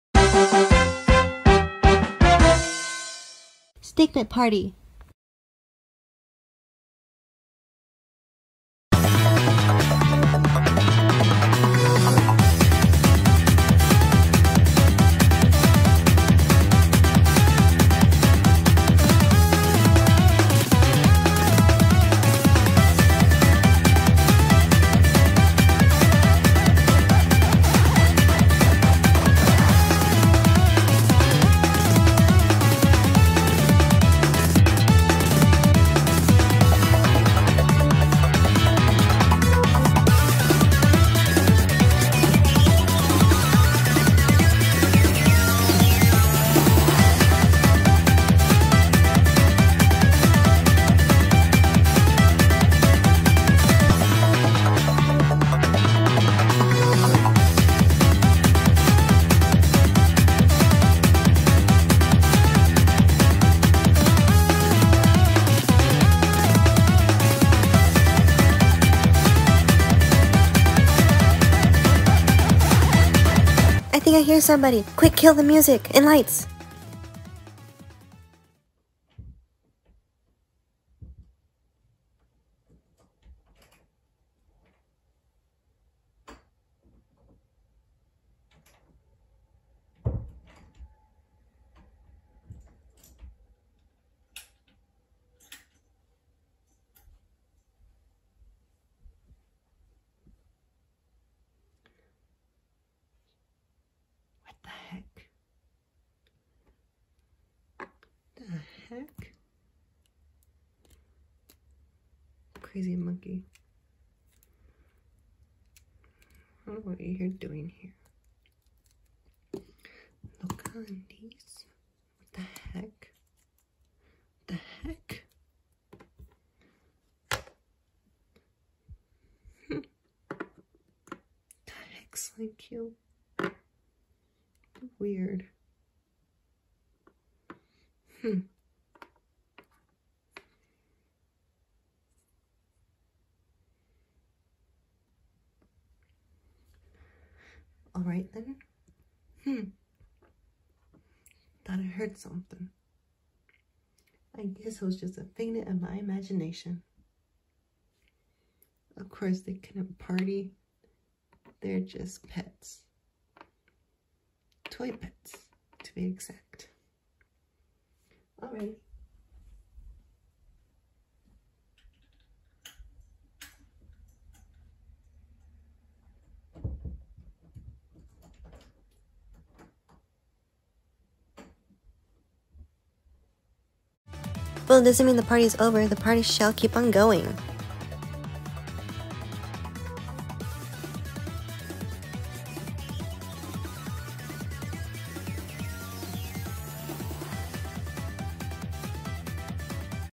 Stikpet Party: The Stikpet Party, Party! Remake!!
We are bringing back the Party and it’s better than ever before and more dance moves as well